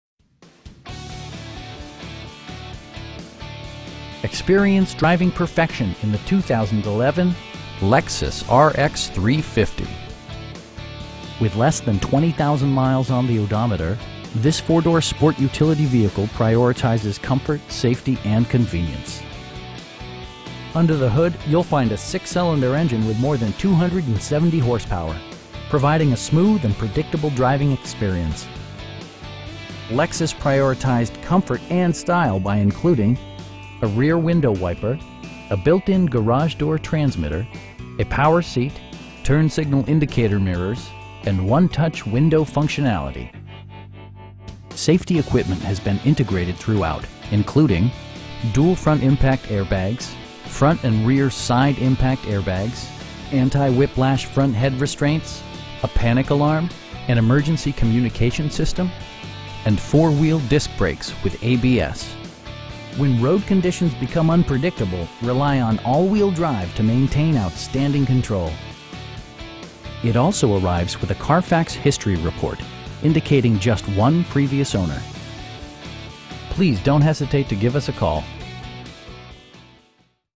2011 Lexus RX 350 in Alexandria, VA 22302
Lindsay Lexus of Alexandria
3410 King St.

Experience driving perfection in the 2011 Lexus RX 350. With less than 20,000 miles on the odometer, this 4 door sport utility vehicle prioritizes comfort, safety and convenience. Under the hood you'll find a 6 cylinder engine with more than 270 horsepower, providing a smooth and predictable driving experience. Lexus prioritized comfort and style by including: a rear window wiper, a built-in garage door transmitter, a power seat, a trip computer, turn signal indicator mirrors, cruise control, and 1-touch window functionality. Safety equipment has been integrated throughout, including: dual front impact airbags, front and rear SIDE impact airbags, traction control, brake assist, anti-whiplash front head restraints, a panic alarm, an emergency communication system, and 4 wheel disc brakes with ABS. When road conditions become unpredictable, rely on all wheel drive to maintain outstanding control. It also arrives with a Carfax history report, indicating just one previous owner. We pride ourselves in the quality that we offer on all of our vehicles. Please don't hesitate to give us a call.